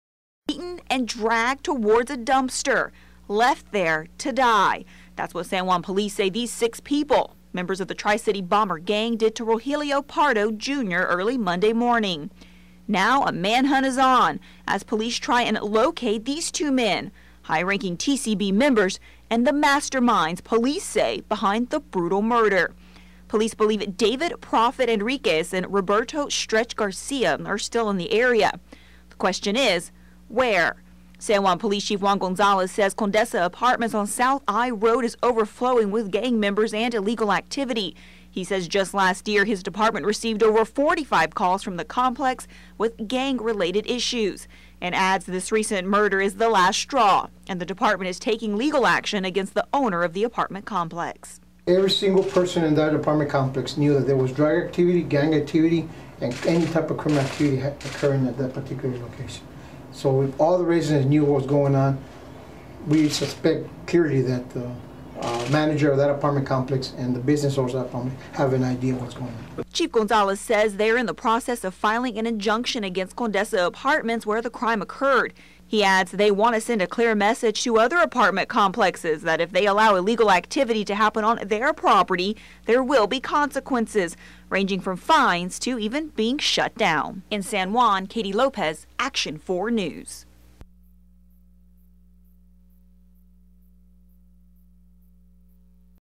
Federal charges possible in San Juan gang member murder
Authorities are considering federal charges following a murder involving members of the Tri-City Bombers gang in San Juan.